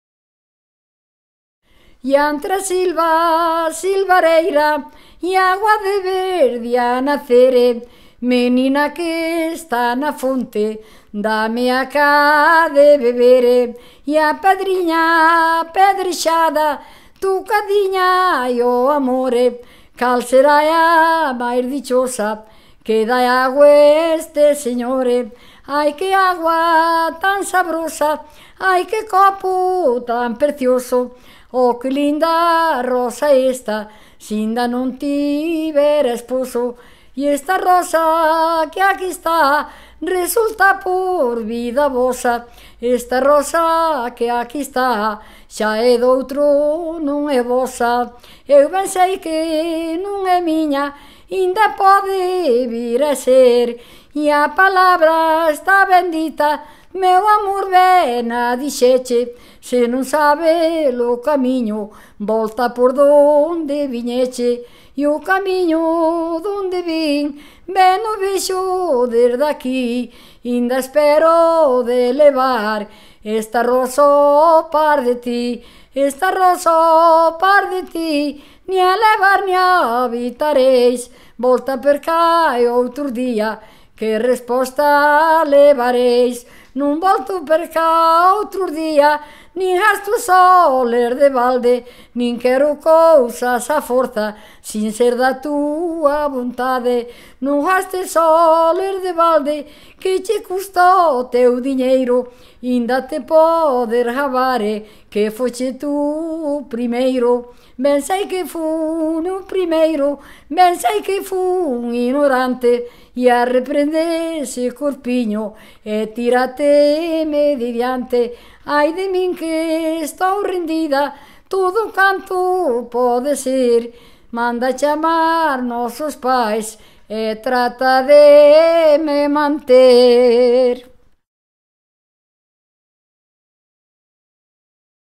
Parrafeo por Felicia de Fofe
Felicia Insuela (Fofe-Covelo, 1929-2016), bota un parrafeo en galego. Gravación feita na súa casa de Paredes (Vilaboa), en xuño de 2013. Arquivo Guillerme Ignacio.

Podcast sobre Felicia de Fofe, na sección O Recuncho da Palleta do programa Lume na Palleira da Radio Galega Música